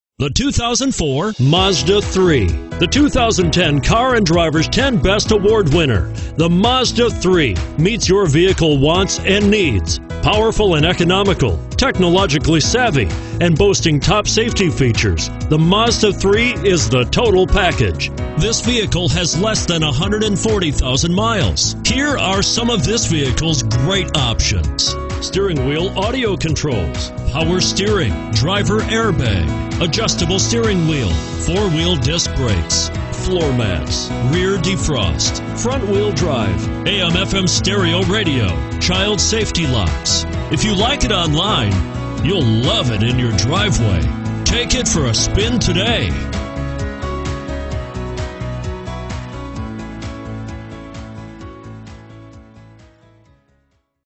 2004 MAZDA MAZDA3 Tulsa, OK E0329060A
2004 MAZDA MAZDA3 Tulsa, OK
Stock# E0329060A

Nelson Mazda  Tulsa
9902 S Memorial Dr.

You'll love this 2004 Mazda Mazda3. This is a car you'll want to take home. With 138433 miles, it features Automatic transmission and an exterior color of Sunlight Silver Metallic. Call us and be the first to open the car door today!